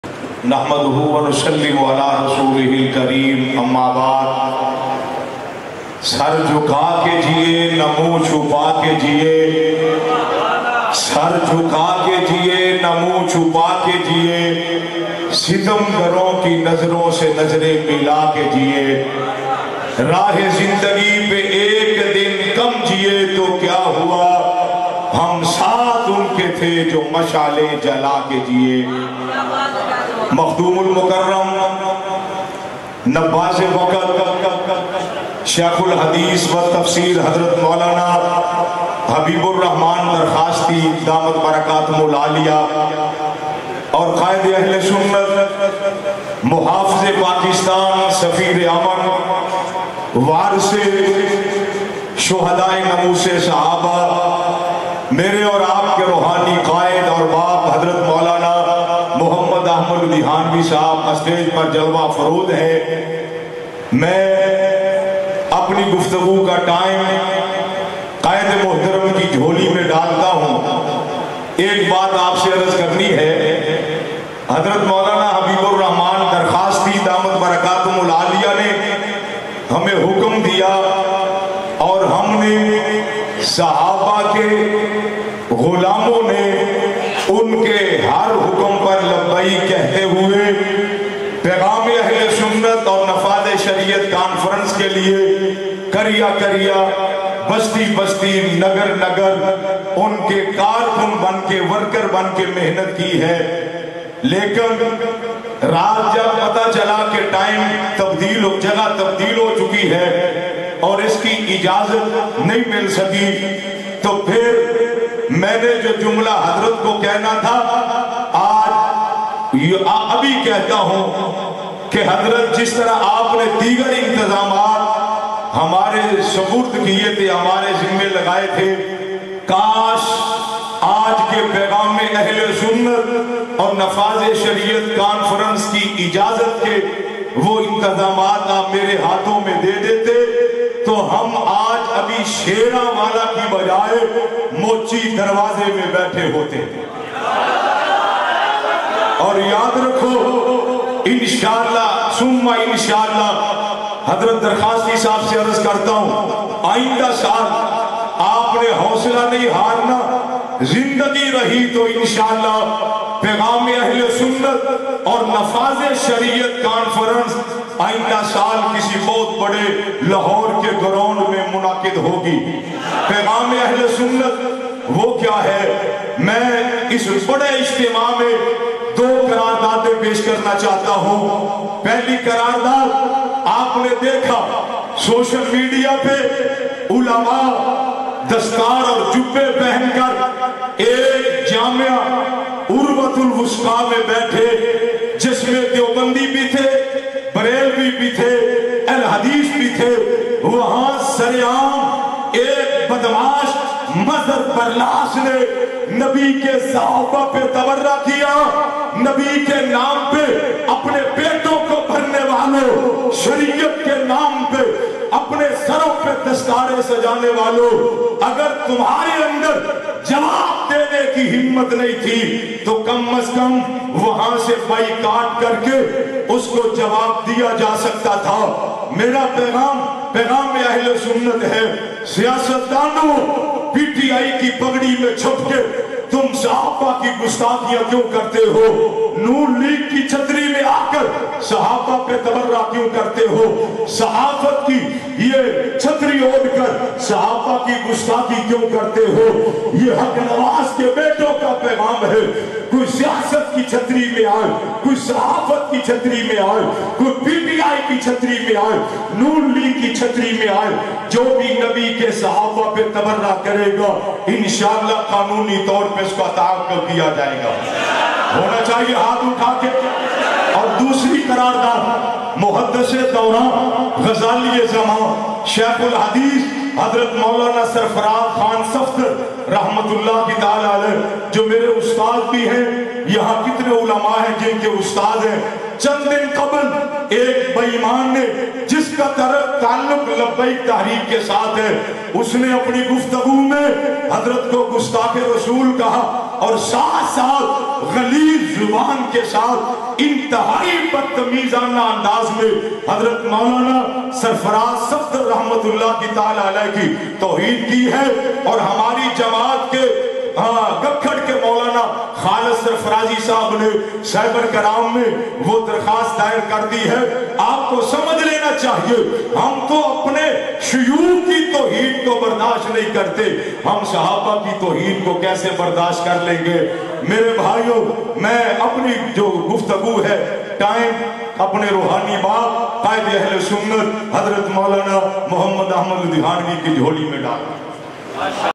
Pegham E Ahle Sunat W Nifaz E Shariyat Canfarns Mulana Ashraf Tahir Sab30-10-2022
بمقام۔موچی دروازہ لاہور